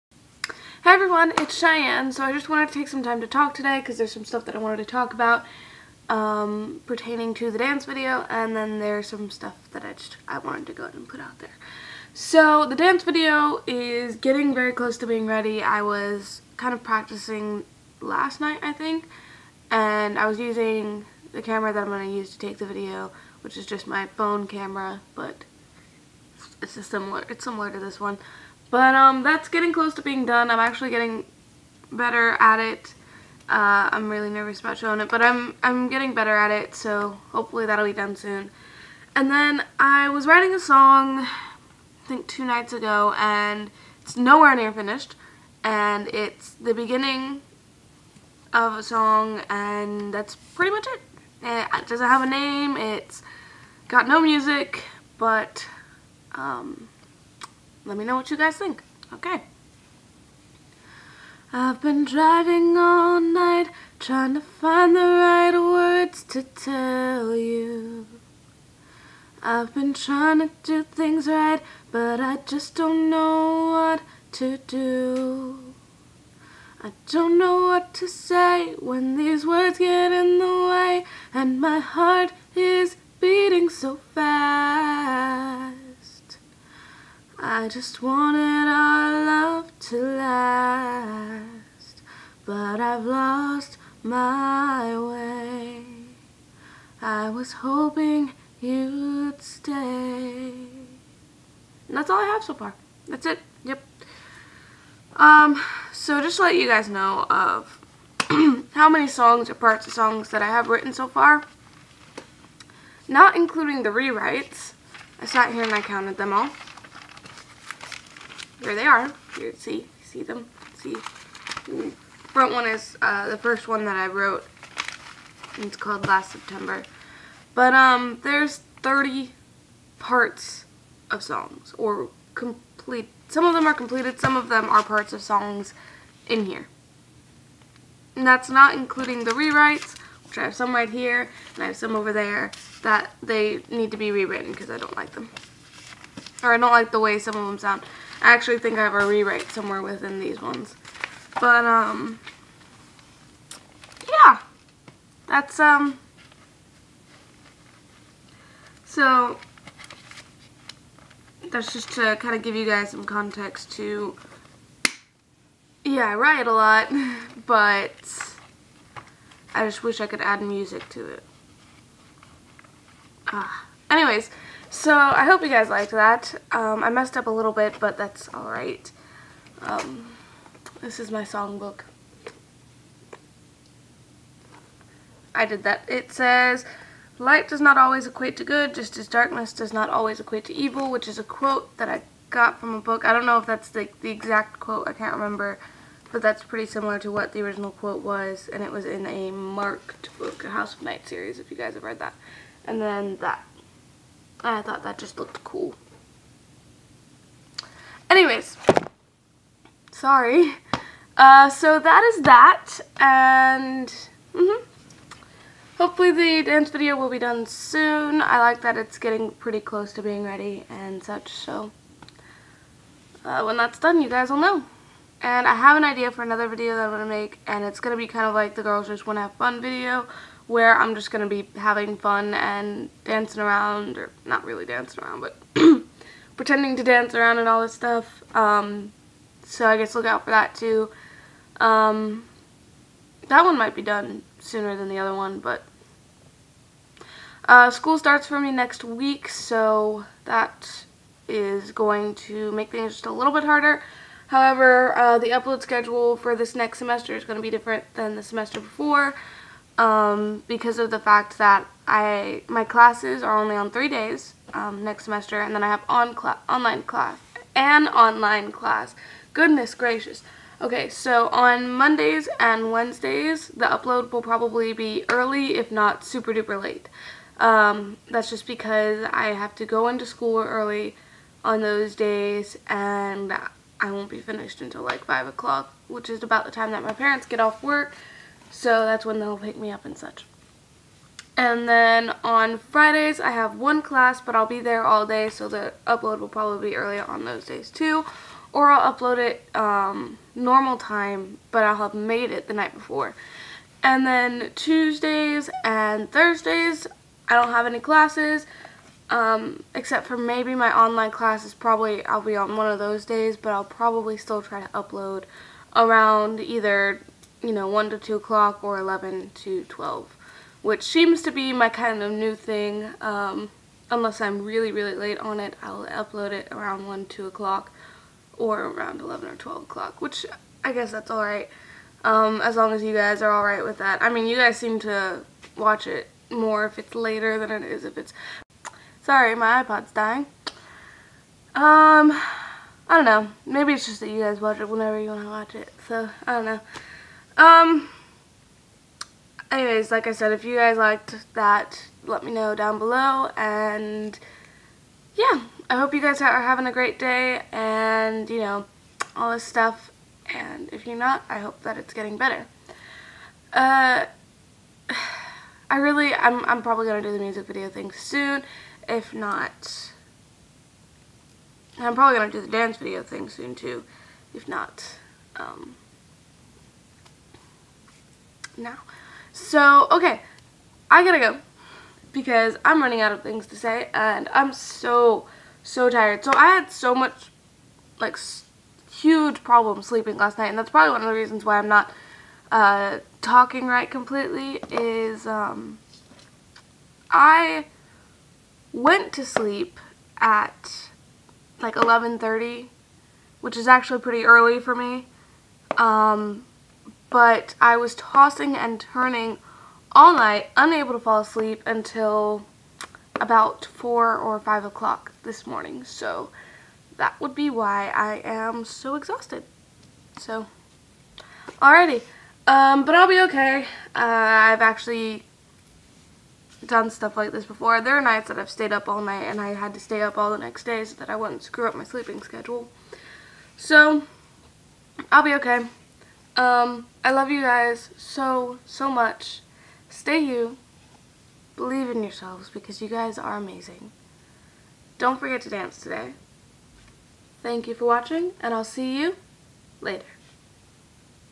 Writing new lyrics. And I'm so tired.
Hey! Just thought I'd give y'all a peak on some lyrics I'm writing and talk about up and coming stuff! I love y'all!